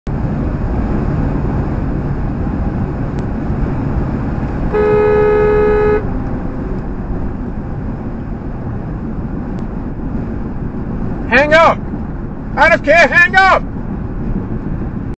Typical Cop driving .New Jersey Garden State Parkway South.
Cop shoots over 4 Lanes of traffic nearly hits me while he's on his phone.After I beep,he hits his strobes,"MG" on the plate IDs this is a Municipal Govt,Veh,Red&Blue Strobes.Exit darting missed his exit.